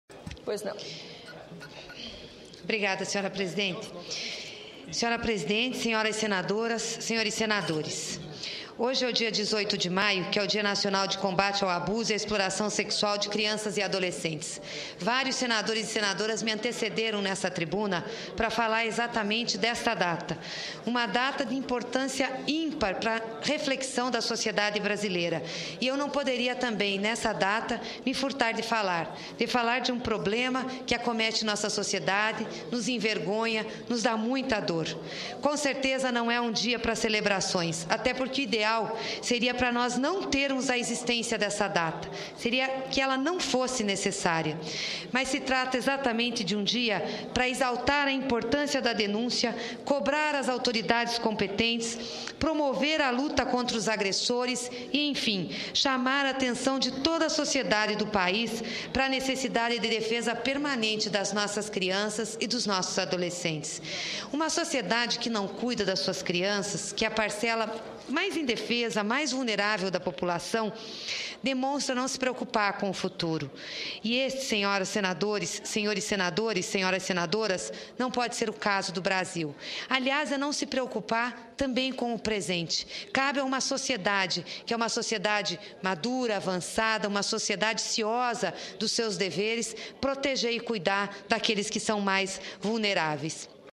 Sen. Gleisi Hoffmann, PT/PR, ressalta importância da denúncia contra a exploração sexual infantil
18/05/2011 - Plenário - Sen. Gleisi Hoffmann, PT/PR, ressalta importância da denúncia contra a exploração sexual infantil